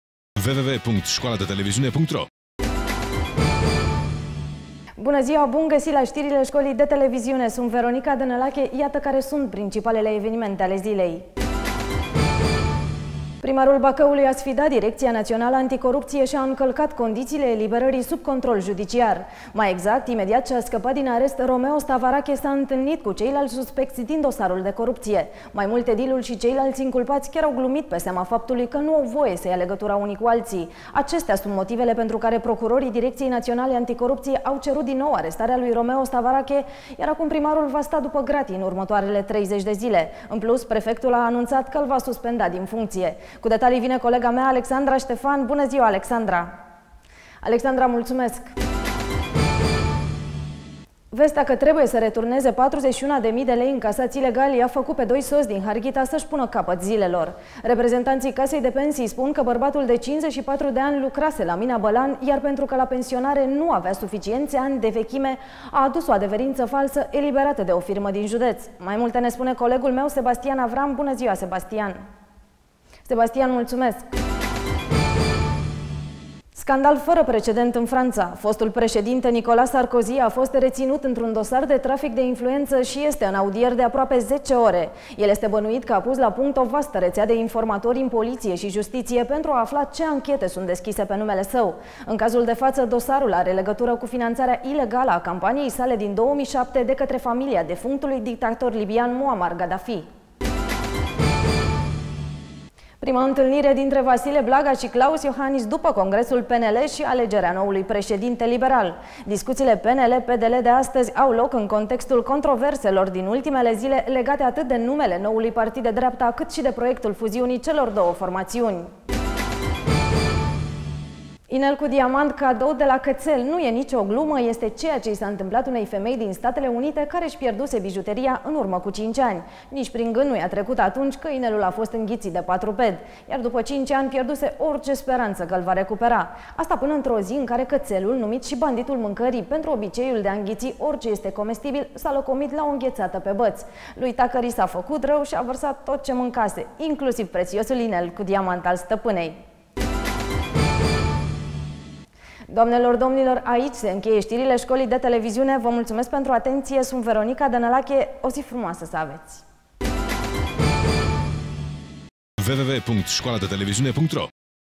Prezentare stiri - Proiect examen 2014
Proiect realizat de Veronica Danalache,  absolventa sectiei Editorial-Artistic  din cadrul Scolii de Televiziune "TUDOR VORNICU". Scoala de Televiziune TUDOR VORNICU este un proiect Media Consulta International si Prima Tv.